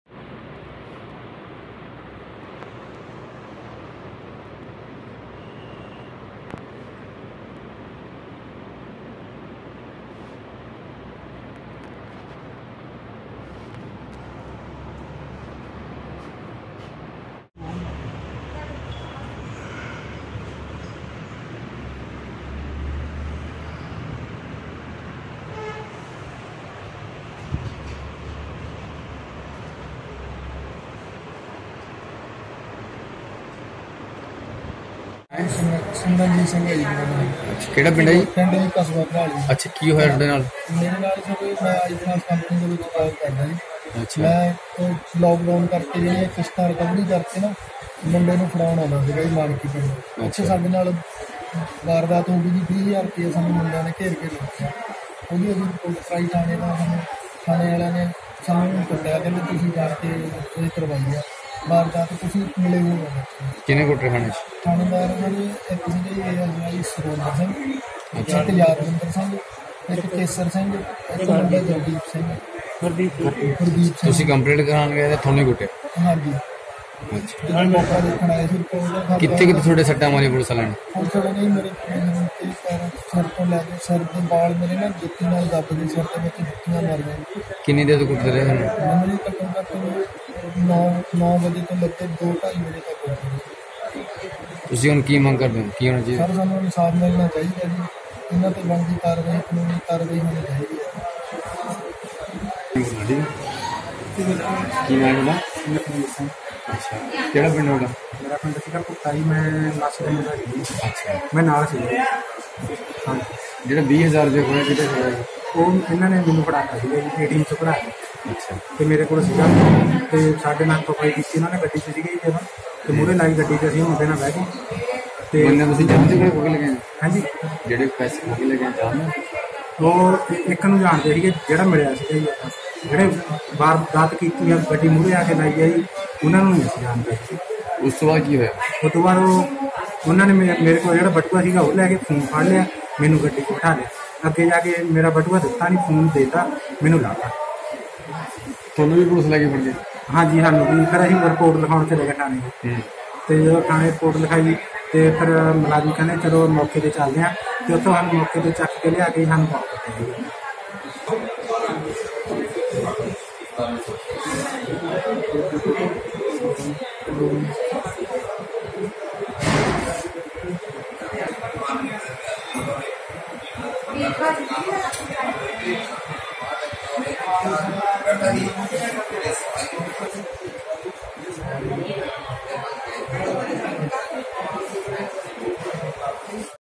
Punjab/ संगरूर पुलिस पर प्रताड़ना के आरोप
Punjab/ संगरूर के गांव ढेपीई के रहने वाले एक केशधारी युवक ने पुलिस पर प्रताड़ना के आरोप लगाए हैं, युवक ने आरोप लगाए हैं कि पुलिस ने उसे बुरी तरह पीटा और उसके केशों की बेअदबी की